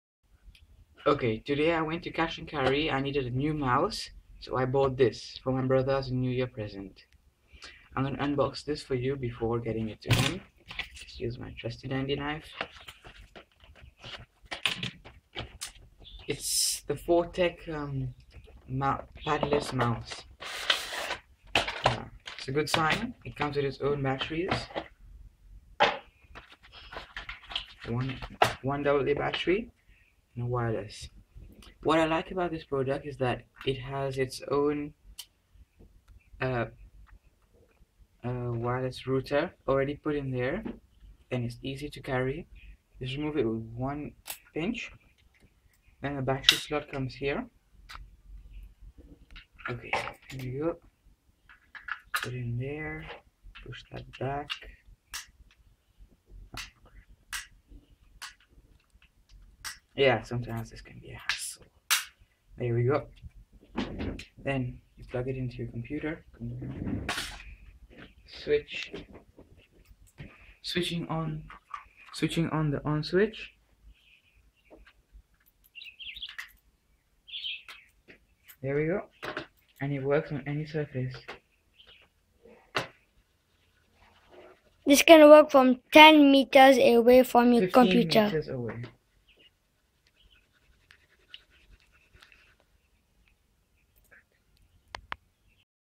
Unboxing 4Tech Paddles mouse (wireless)
unboxing the 4tech paddles mouse as a new year present for my brother. unboxing + review